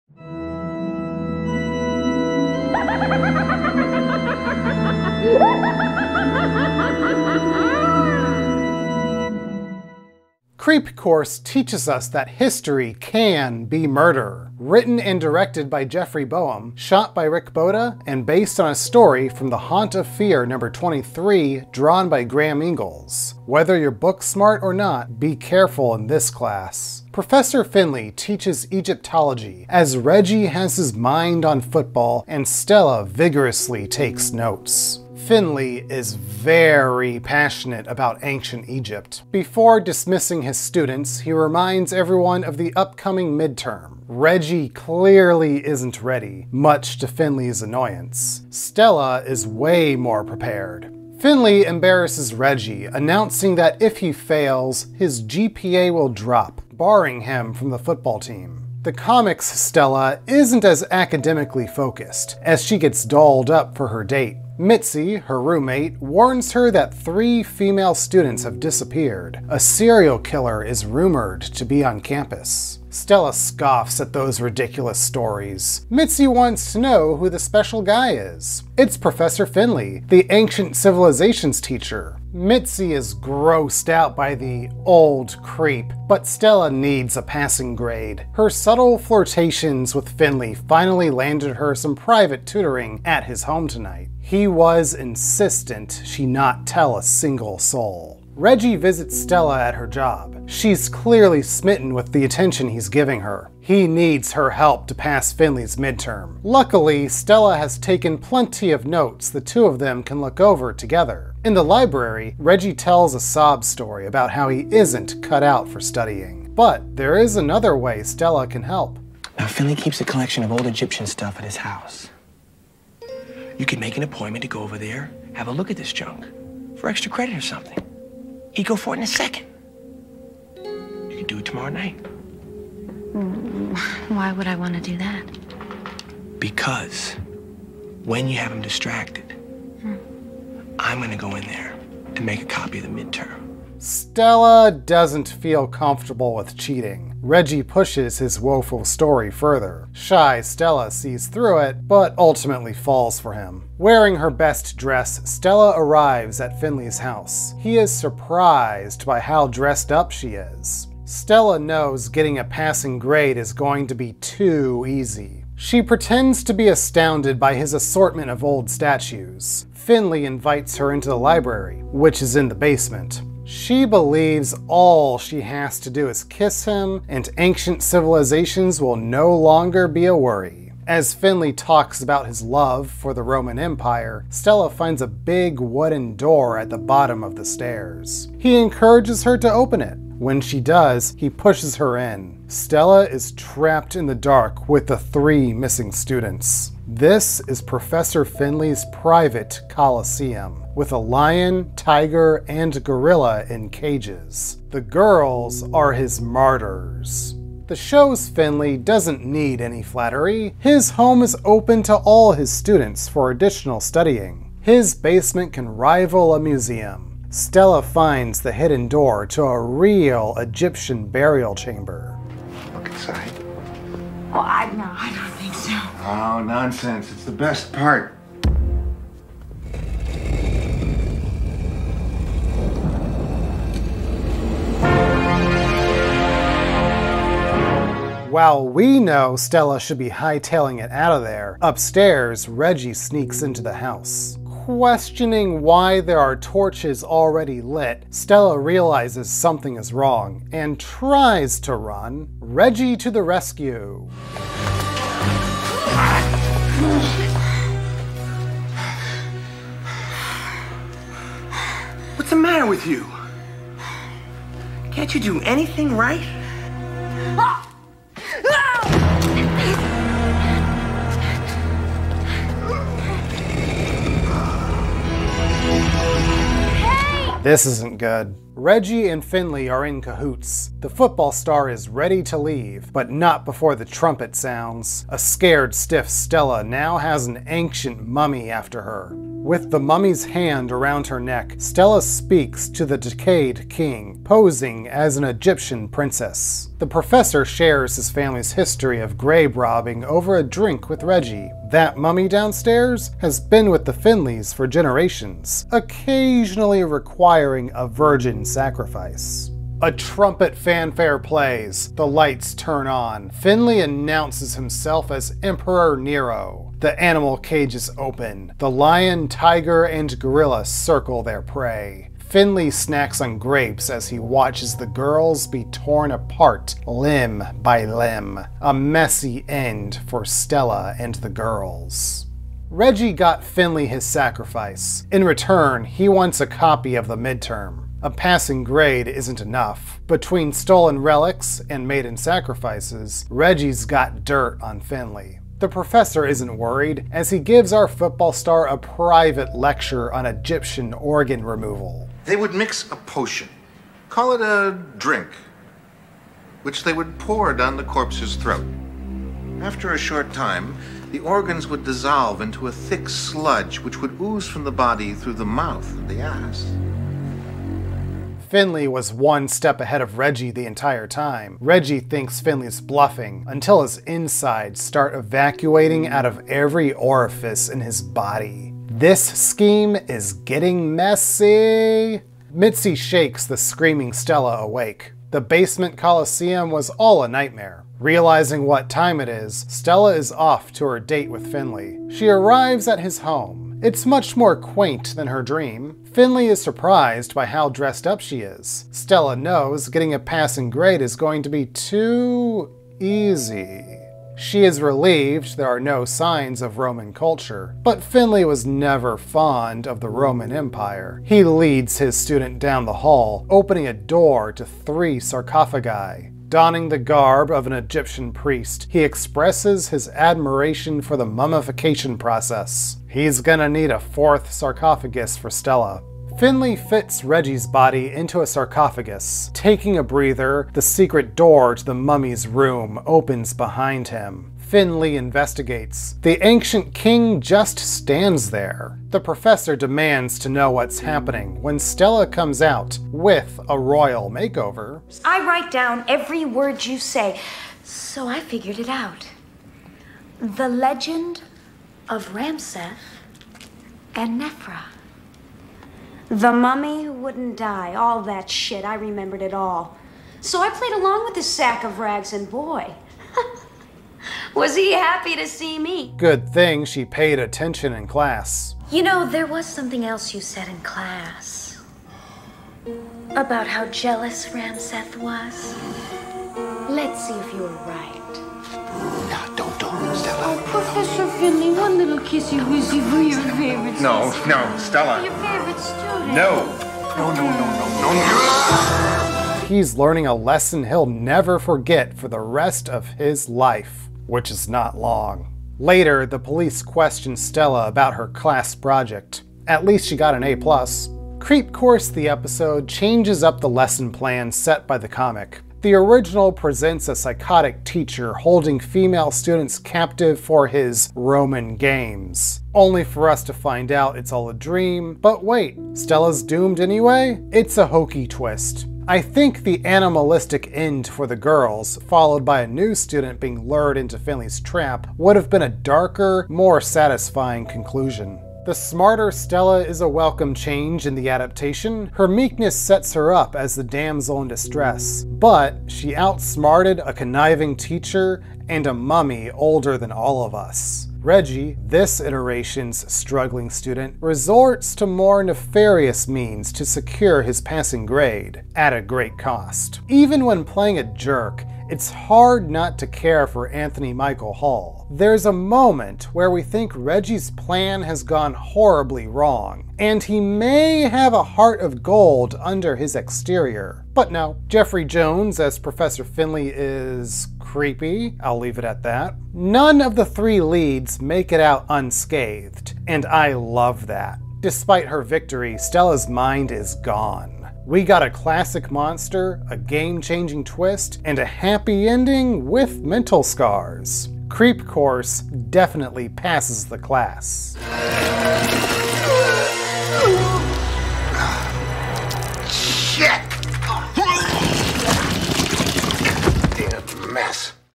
Crypt Keeping: Season 5, Episode 9 - Creep Course
A great history teacher can make Ancient Egypt and the Roman Empire come alive! A crypt teacher will make you live them! Anthony Michael Hall, Jeffrey Jones, and Nina Siemaszko takes us back to college this week for a "Creep Course."

Tell me what you think about Tales from the Crypt and Creep Course in the comments!